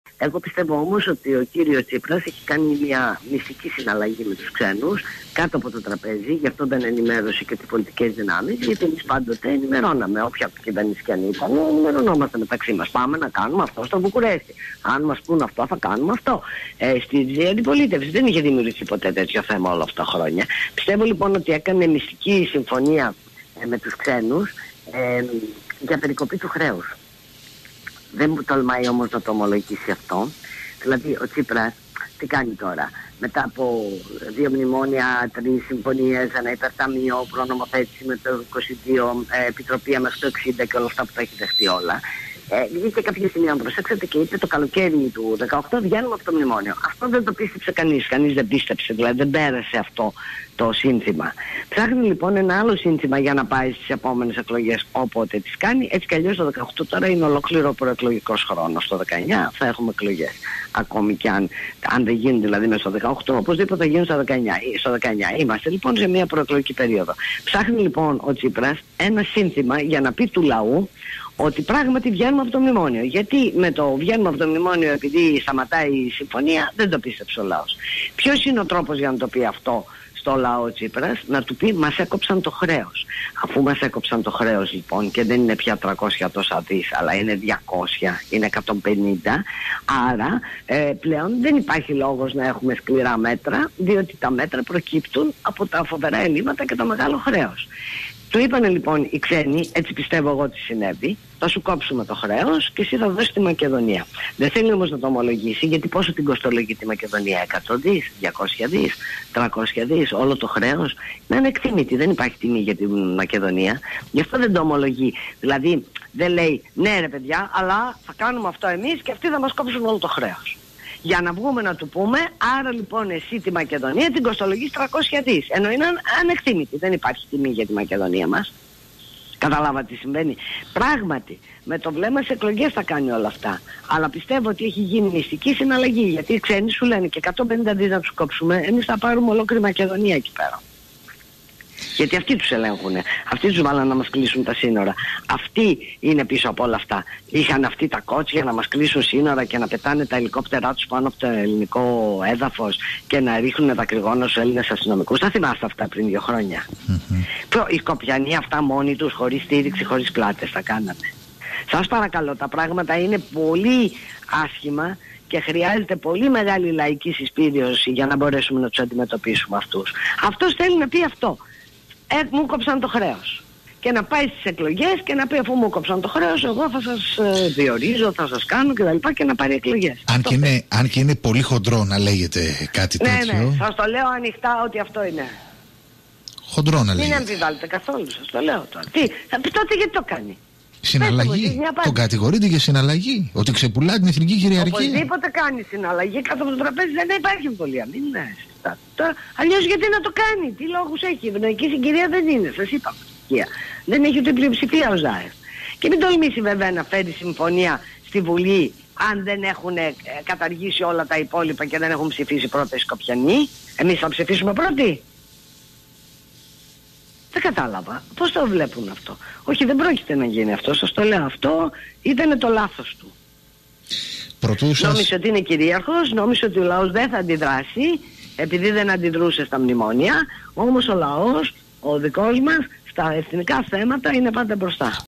ΔΗΛΩΣΗ ΣΟΚ Βούλτεψη για Τσίπρα: Έχει κάνει μυστική συναλλαγή για Μακεδονία και χρέος
Συνέντευξη πρώην υπουργού και νυν βουλευτή ΝΔ, Σοφίας Βούλτεψη στο Radio Marconi 96,1 F.M. και τον Αντώνη Κουμιώτη (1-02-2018)...